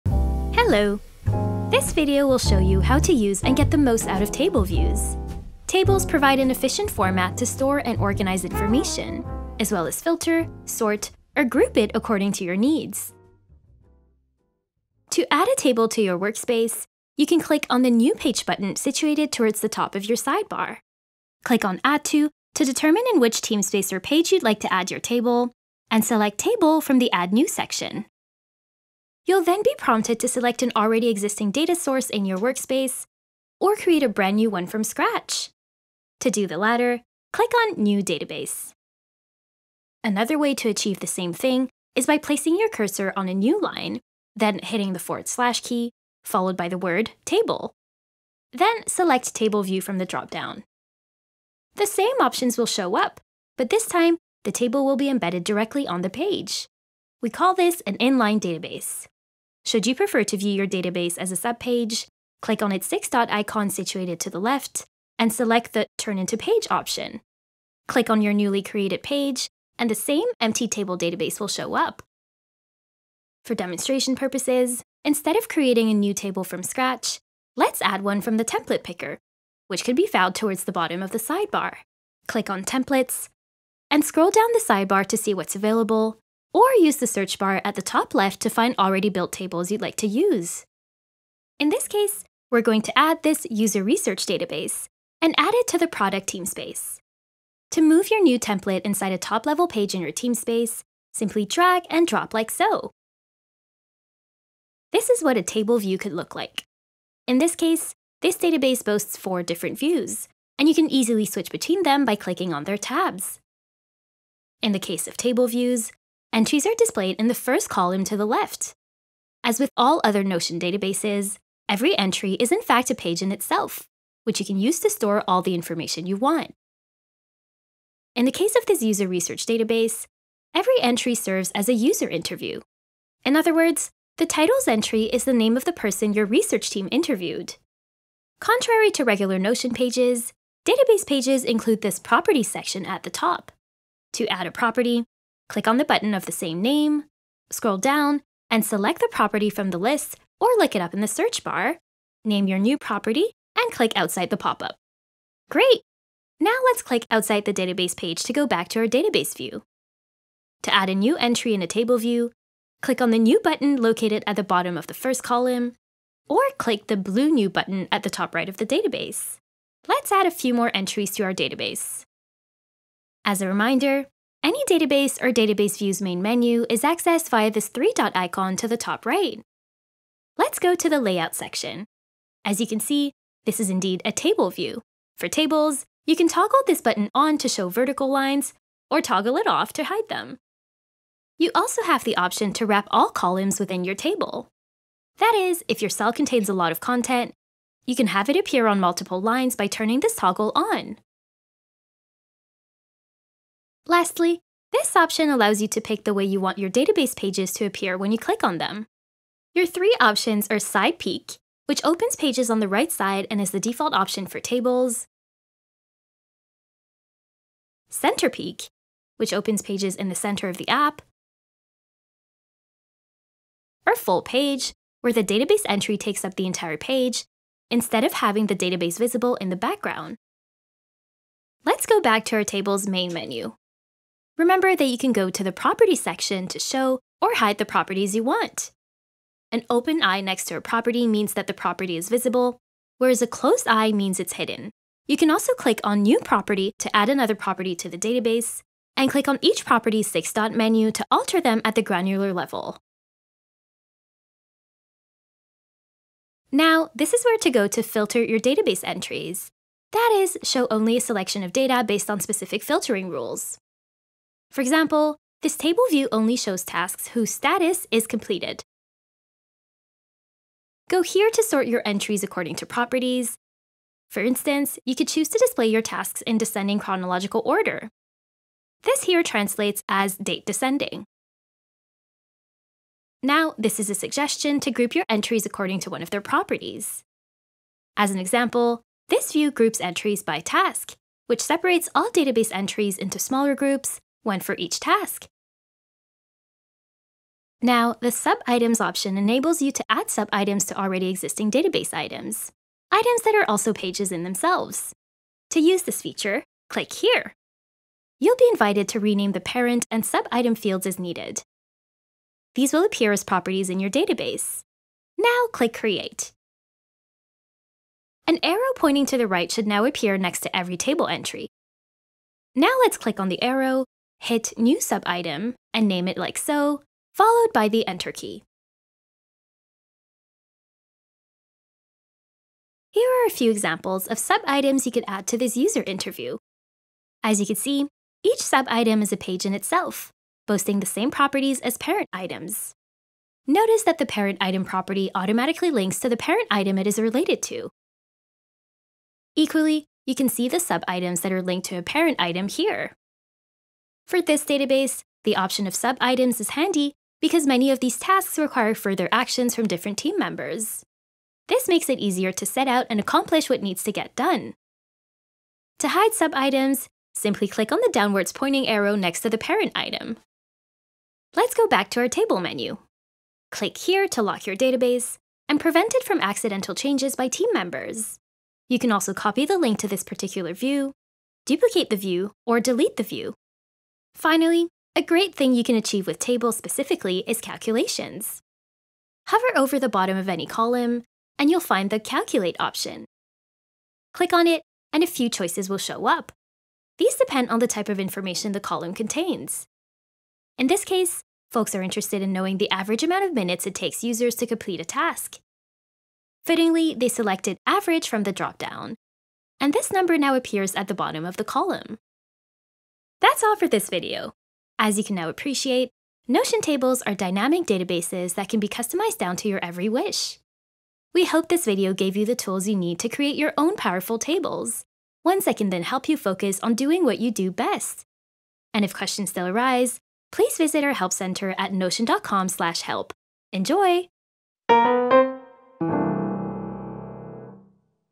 Table views
Notion tables are dynamic databases that can be customized down to your every wish. They provide an efficient format to store and organize information, as well as filter, sort, or group it according to your needs. This video will show you how to use and get the most out of table views.